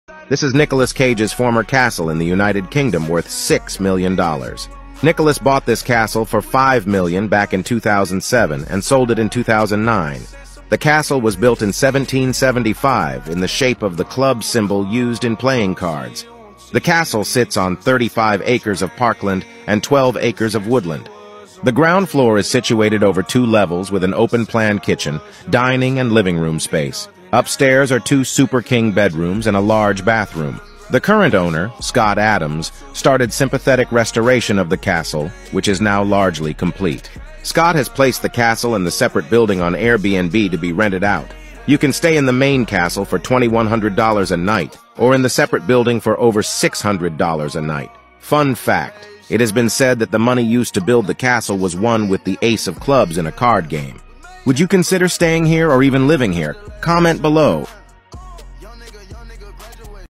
Castle that once belonged to Nicolas Cage available for holiday let for a whopping £1,650 per night
An impressive castle that once belonged to actor Nicolas Cage is now available to stay in for a whopping £1,650 per night.

Midford Castle is a Grade I Listed building in Midford, Somerset and dates back to 1775, when it was built for Henry Roebuck in the shape of the clubs symbol used in playing cards.

The property was purchased by Cage in 2007 for around £4.75m but Cage, 59, subsequently sold it to businessman Scott Adams just two years later for a fee believed to be around £3.5m.

Following years of restoration work, the castle and buildings in its grounds are now available to let on Airbnb for various prices - with the priciest option working out at a four-figure sum per night.